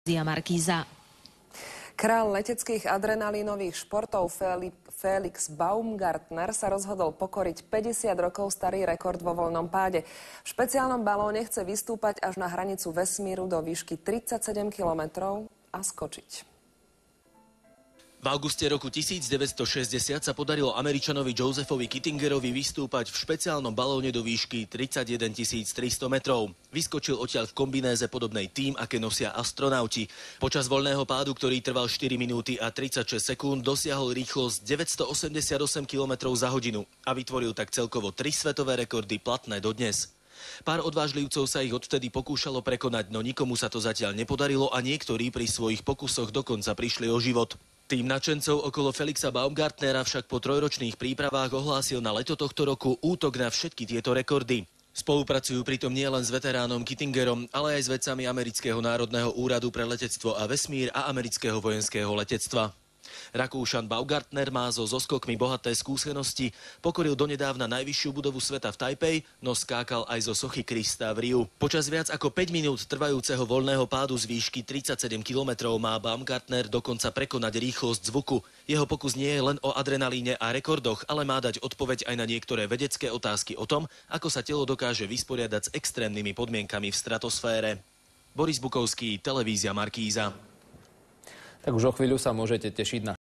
Red Bull Stratos on TV Markíza
Pred troma rokmi sa stretol pilot Felix Baumgartner (40) s plukovníkom U.S. Air Force Josephom Kittingerom, ktorý v roku 1960 uskutočnil prvý úspešný skok zo stratosféry z výšky 31,3 kilometrov. Ihneď po stretnutí sa Felix spolu s tímom vesmírnych odborníkov pustil do náročnej misie - Red Bull Stratos. Prekonať vlastným telom, ako prvý človek vôbec, rýchlosť zvuku! Baumgartner má v pláne vystúpať do výšky 36,576 kilometrov nad zemský povrch, do najvyšších vrstiev stratosféry, v kapsule nadnášanej héliovým balónom. Jeho telo bude chrániť špeciálne vyvinutý skafander. Zo stratosféry následne skočí voľným pádom, pri ktorom dosiahne rýchlosť až 1100 kilometrov za hodinu a pomocou padáka pristane na zemi. Ak bude misia Red Bull Stratos úspešná, nielen že prekoná štyri svetové rekordy, ale získané dáta posunú ďalej doterajšie výskumy v oblasti atmosférickej bezpečnosti a letov ľudí do vesmíru.

,,Päťdesiat rokov sa ľudia snažia prekonať môj rekord a niektorí pri svojich pokusoch zahynuli, povedal Joseph Kittinger. ,,Ale verím, že s našimi unikátnym vybavením, mimoriadnym tímom odborníkov a Baumgartnerovými jedinečnými schopnosťami a húževnatosťou bude misia Red Bull Stratos úspešná.

,,Je to naozaj skok do neznáma. Nikto nedokáže predvídať, ako ľudské telo zareaguje pri nadzvukovej rýchlosti, povedal Felix Baumgartner. ,,Musíme to však zistiť. Budúce vesmírne výskumy potrebujú nájsť spôsob, ako budú môcť kozmonauti vyskočiť z veľkej výšky v stave núdze.